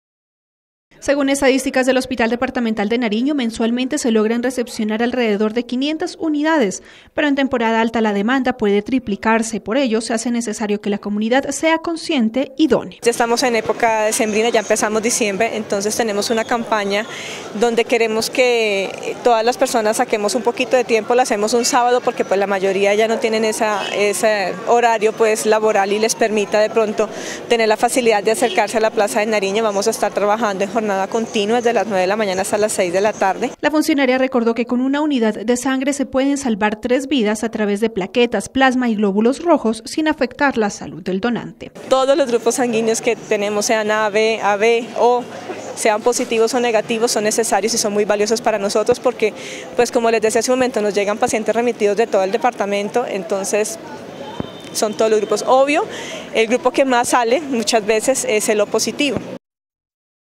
CNC Noticias Pasto - Se requiere mas unidades de sangre
Para temporada de fin de año, se requiere con urgencia de más unidades de sangre en los bancos de los hospitales, para este fin de semana el Hospital Departamental adelantará una jornada especial de donación